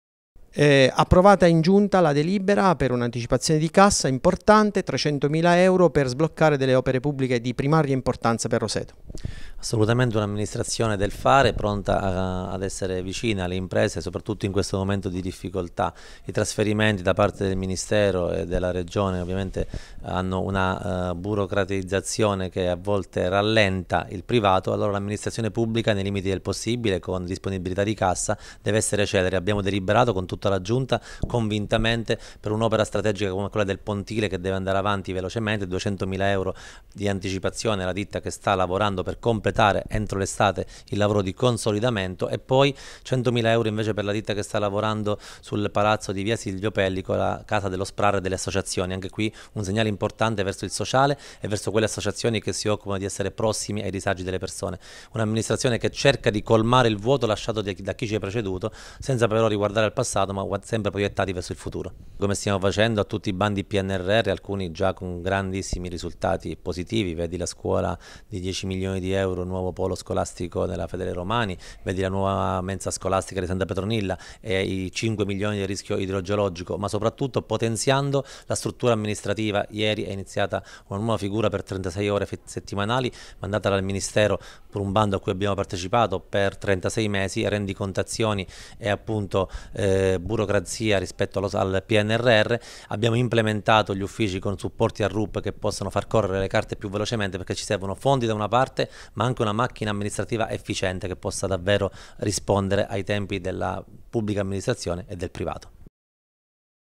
Roseto riprendono i lavori per due opere pubbliche
La giunta guidata da Nugnes, con un atto di anticipo di cassa, sborserà 300mila euro in totale, di cui 200mila per il pontile e 100mila per il centro di accoglienza migranti. Ripartiranno così i lavori, consentendo alle ditte appaltatrici di proseguire il completamento delle due infrastrutture, ritenute strategiche dalla maggioranza che governa la città.